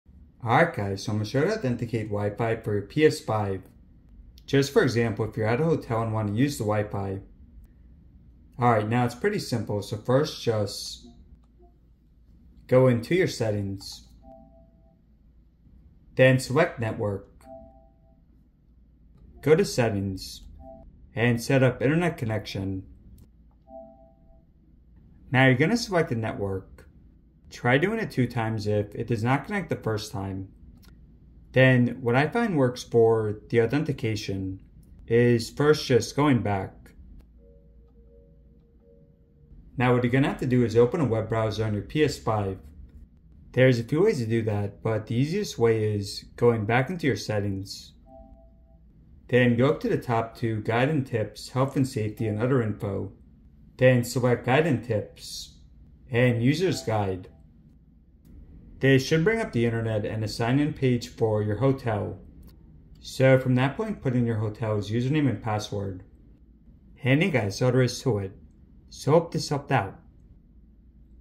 PS5 / PS5 Slim - How to Authenticate WiFi
How to authenticate WiFi on PlayStation 5 Slim (or PS5 Pro) if not connecting or showing. For instance, if you're at a hotel.